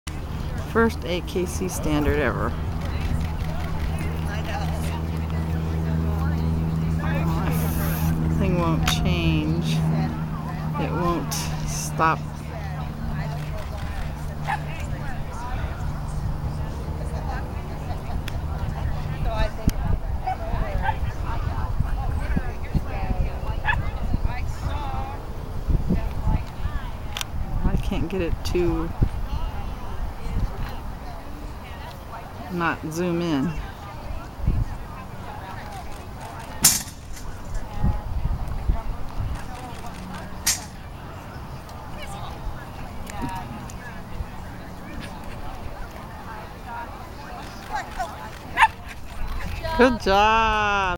Jill & Ziva first AKC Standard
January 30, 2016.  Paradise Valley Park, AZ.  Jill and Ziva first AKC Standard.  Perfect run except weave poles!